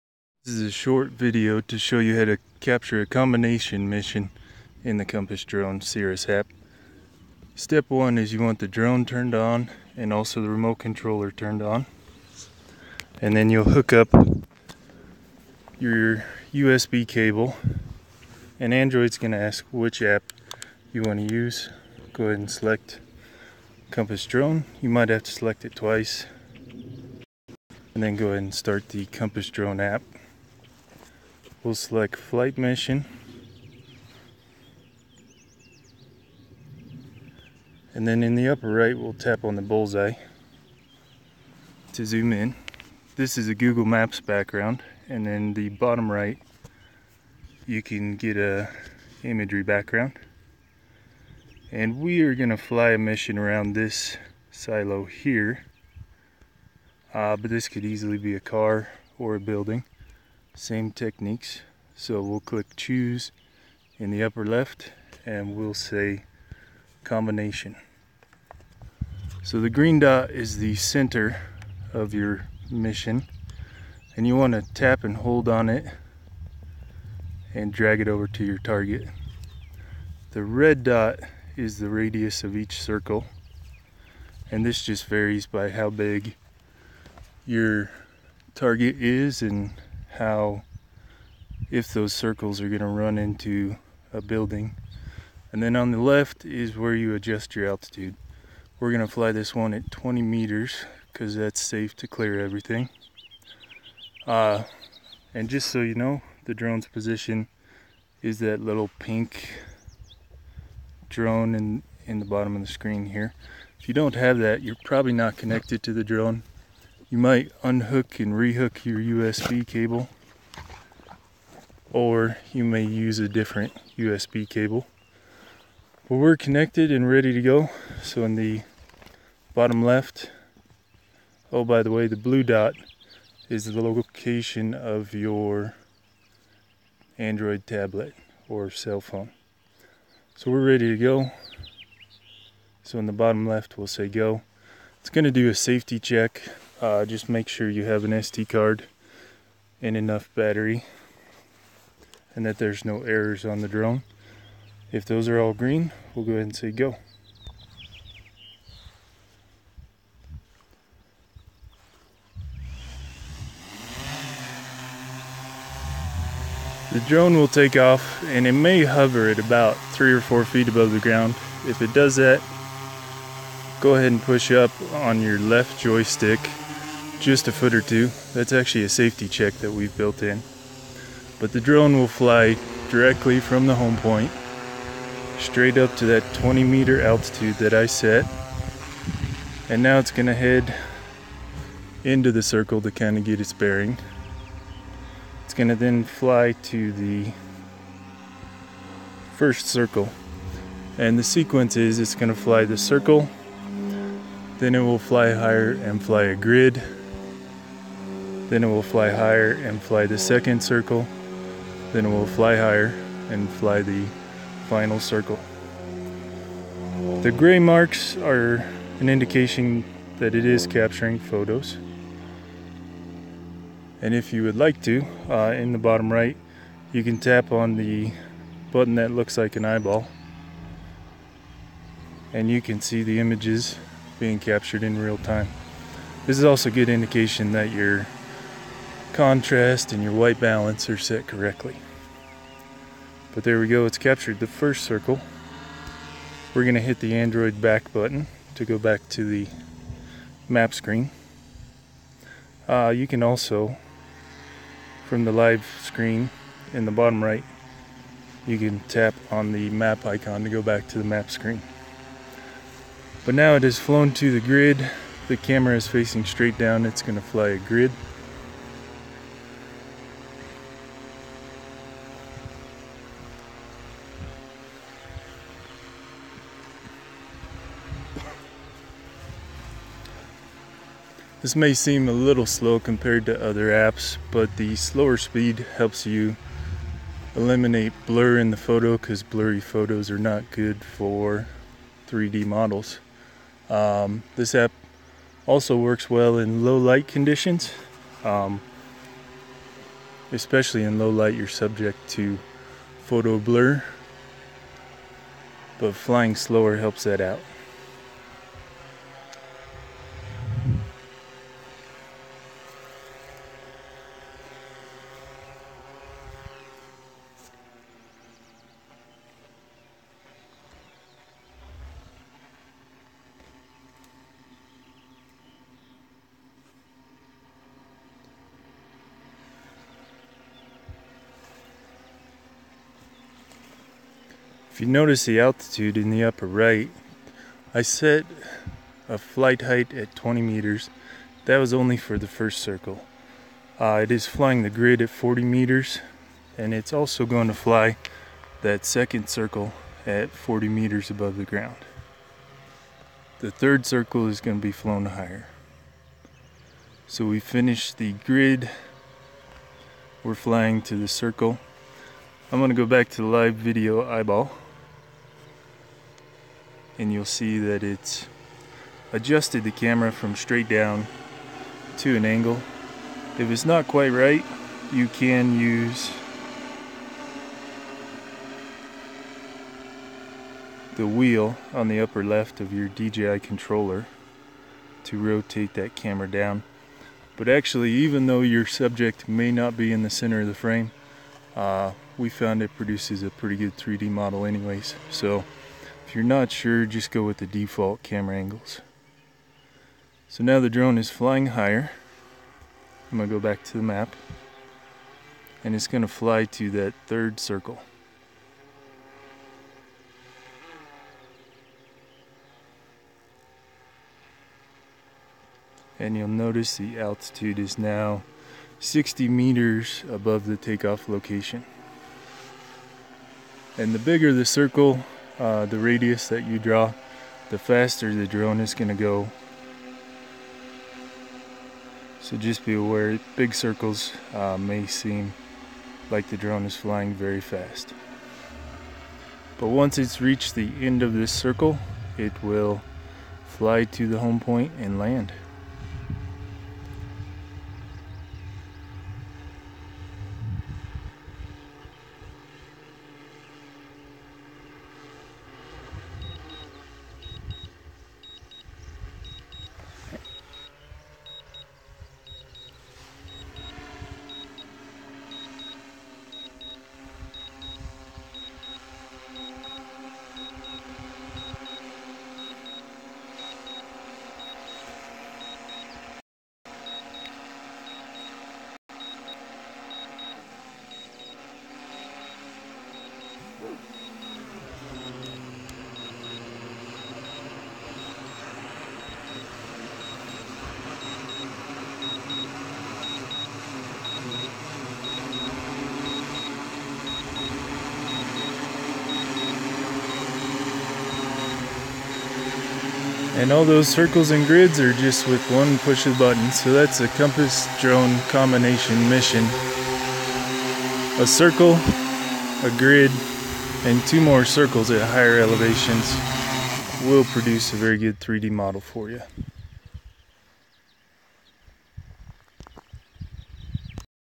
CIRRUAS Combination Mission
Quick overview of what to expect with the CIRRUAS Combination Mission